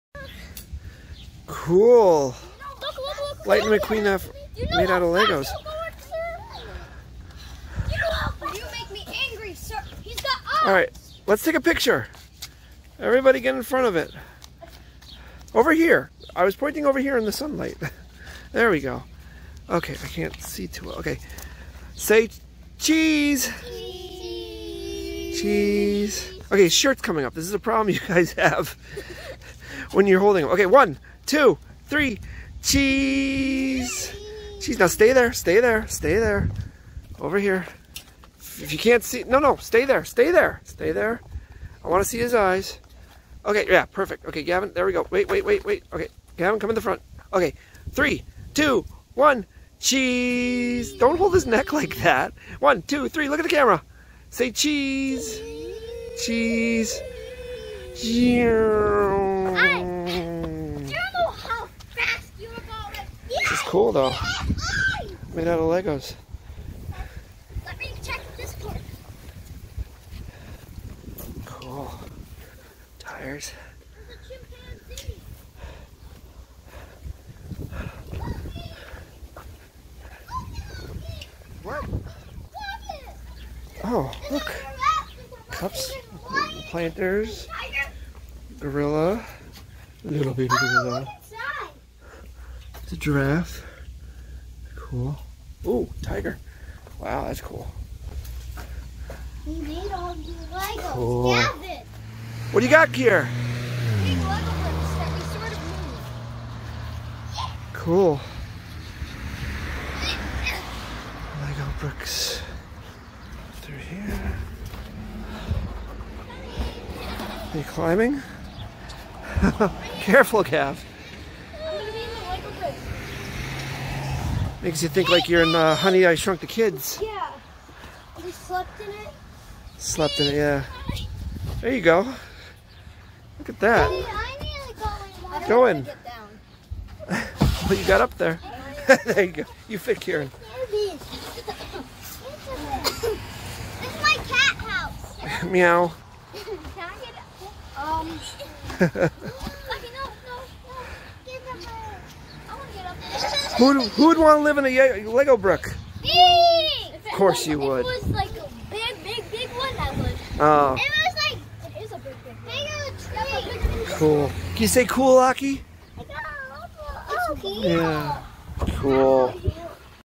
1,360 - Lego Playground (Lego Lightning McQueen)
We have a local Lego Factory, and they have a small playground at their Children's Discovery Center. There they have a lifesize Lightning McQueen in Legos! ...